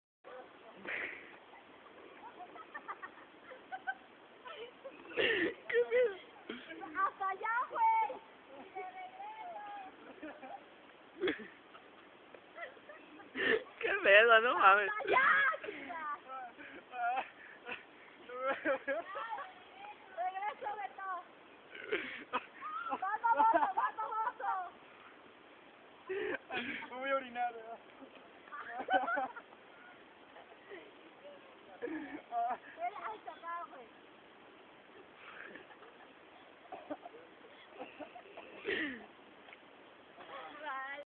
RETOS FRAY SERVANDO ALBECHE ESTÚPIDO
PS ESTO FUE CERKA DEL METRO FRAY CERVANDO EN EL PARKE DE LOS PAPELEROS O ALGÚN PEDO ASÍ

SE RIFÓ MI VALEDOR

LA NETA SE VE RE-ESTÚPIDO

SALE